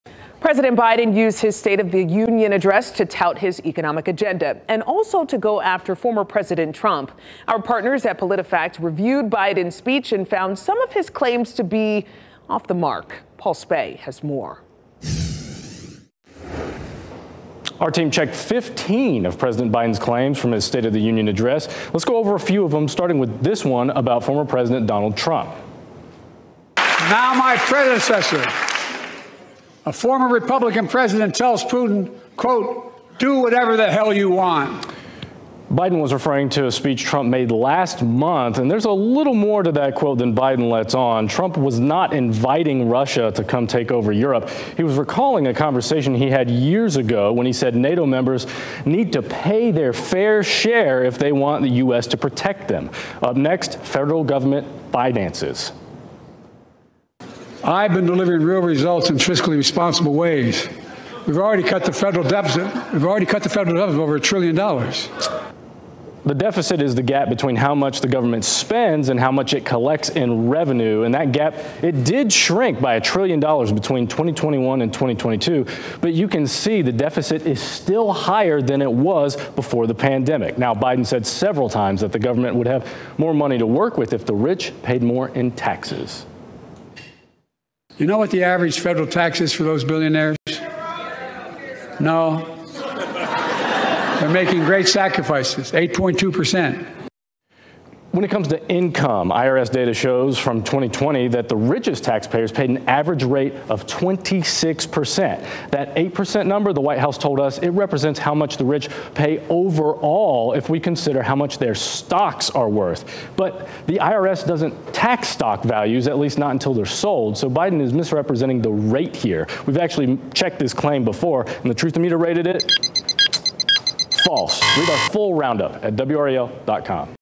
Fact-checking Biden SOTU claims about Trump and the economy
Facing a challenging path to reelection amid low favorability ratings and public wariness over the economy, President Joe Biden used his 2024 State of the Union address to take a fighting posture.